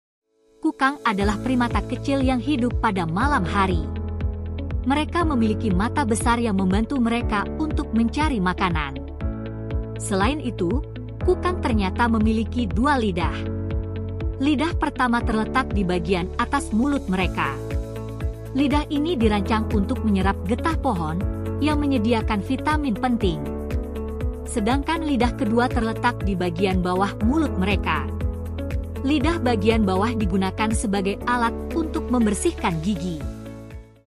Kukang Memiliki Dua Lidah
Kukang memiliki dua lidah didalam mulutnya. Lidah pertama terletak diatas digunakan untuk menyerap getah pohon. Sedangkan lidah ke dua terletak dibagian bawah berfungsi untuk membersihkan gigi.